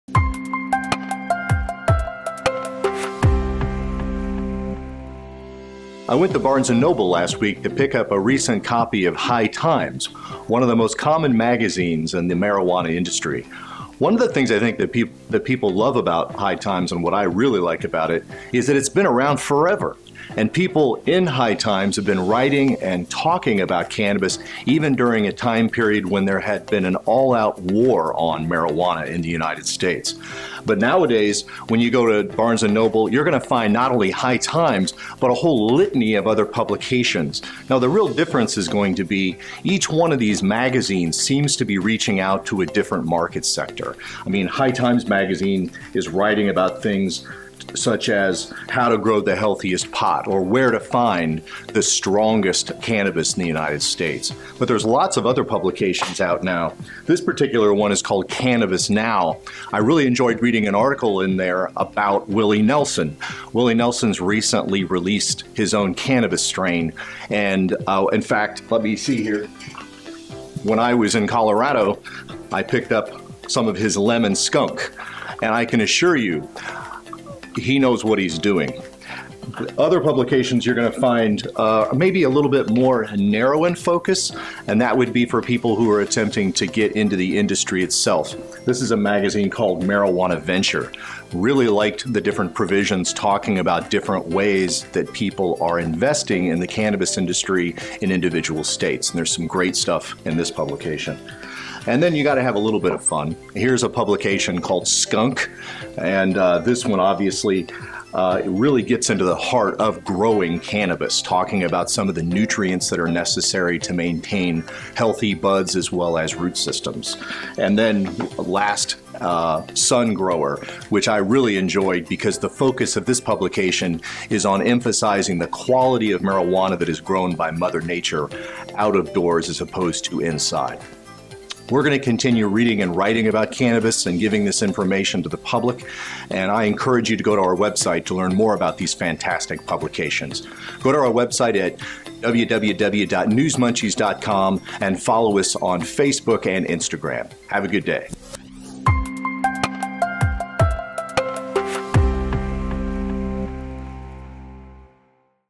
Cannabis Industry Embraced By Publishers
With a majority of states legalizing the cannabis plant for either medicinal or recreational usage, it’s important to keep up on changes in the cannabis industry. Learn about the cannabis industry by consulting these magazines, media outlets, and informational resources for relevant news, political happenings, cannabis legislation, horticultural advancements, economic trends, and medical information.